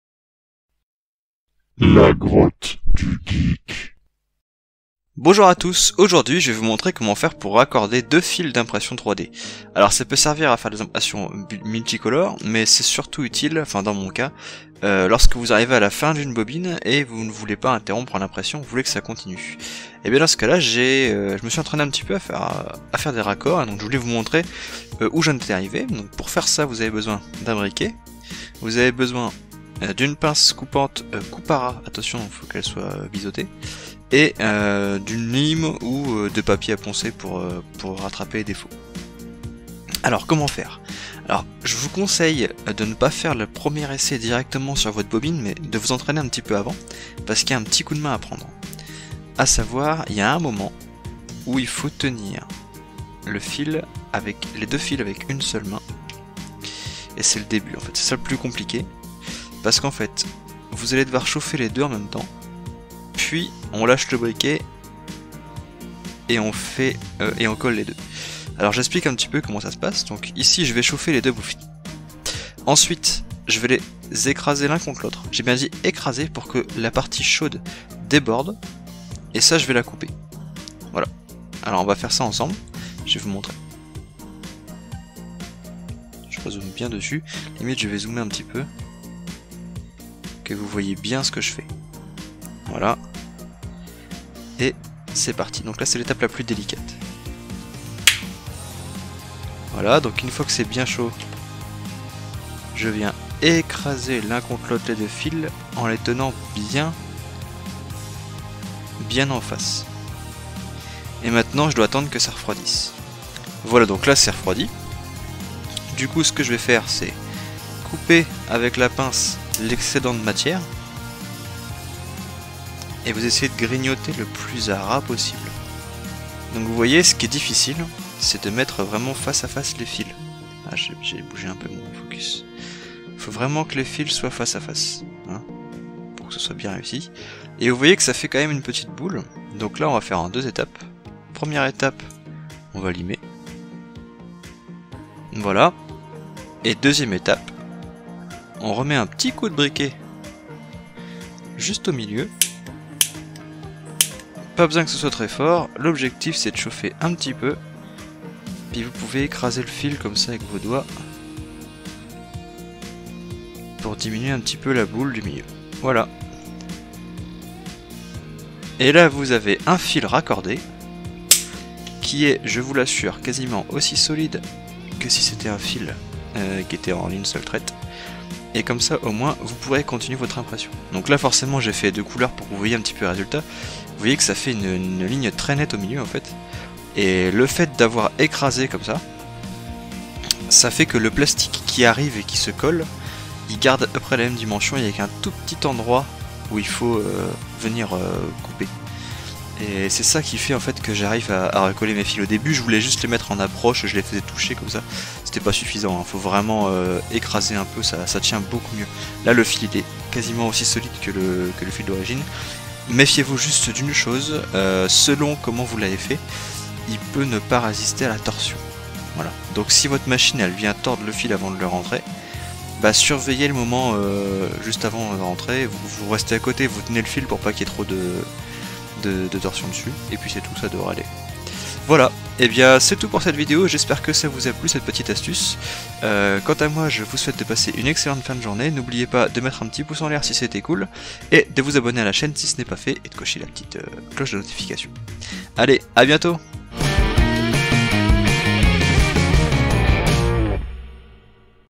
Ma technique pour raccorder 2 fils d'impression 3D
Au fil des impressions, j'ai fini par trouver une technique pour raccorder mes fils qui fonctionne bien et qui n'est pas trop compliquée.
Voilà enfin de quoi finir les fonds de bobines ;)

Visitez mon magasin sur Etsy